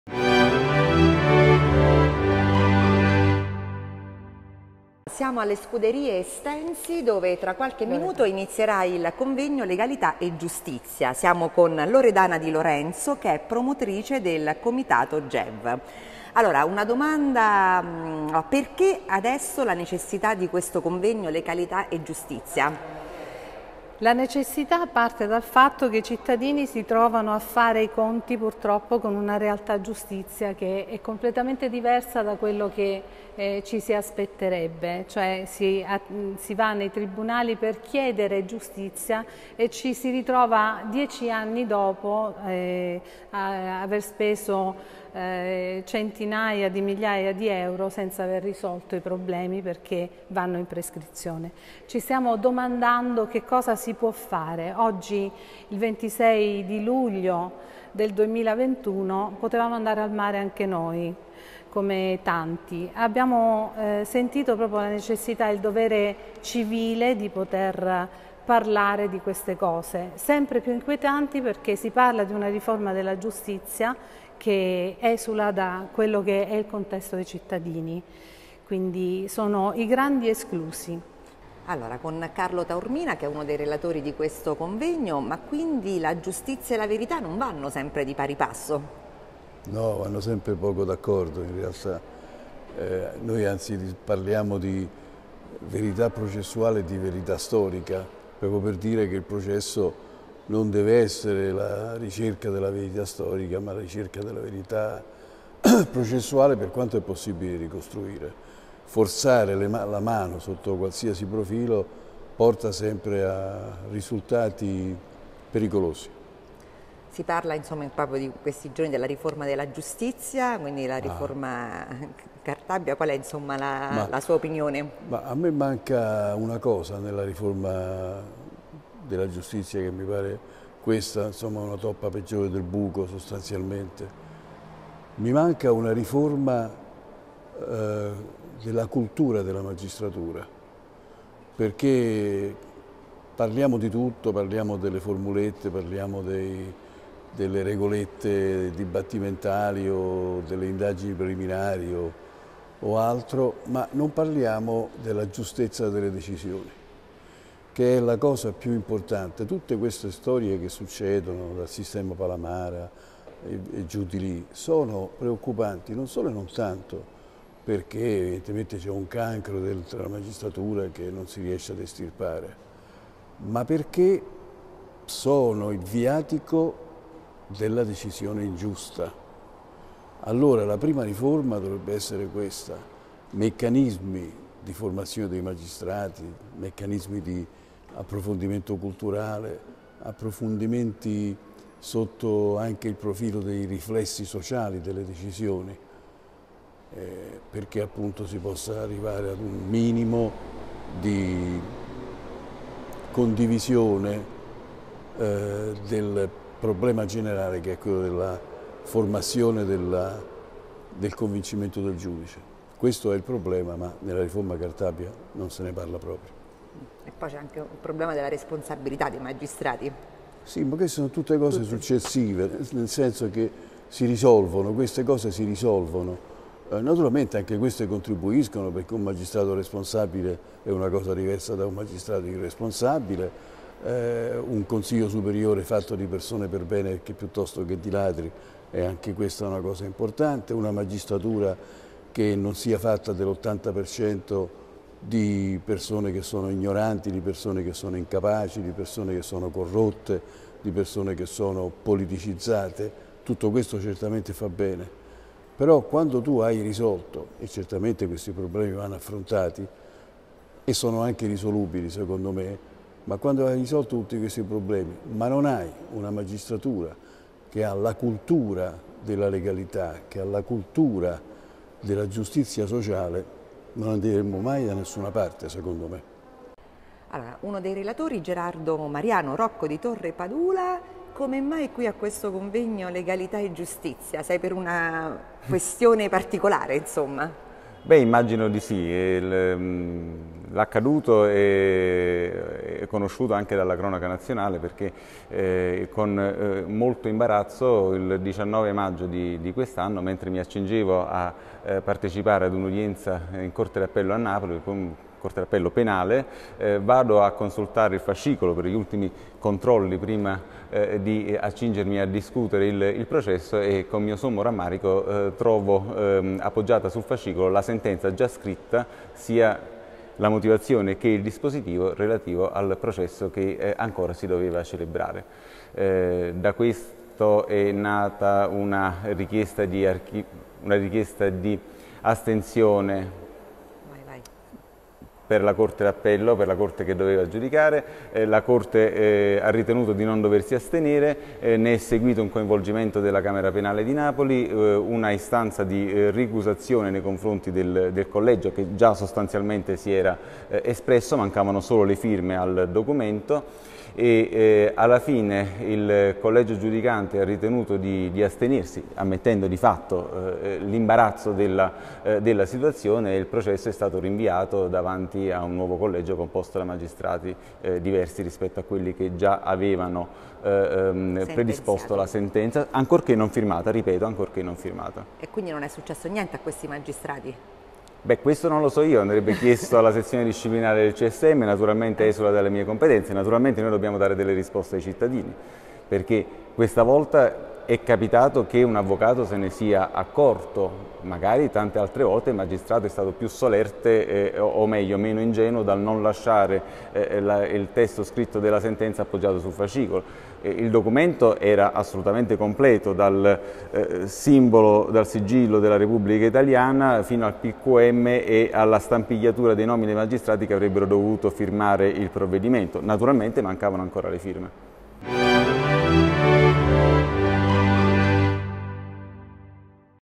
A Tivoli il Convegno Legality and Justice Day
All'incontro, organizzato dal Comitato Giustizia e Verità, Gev, ha partecipato anche Carlo Taormina.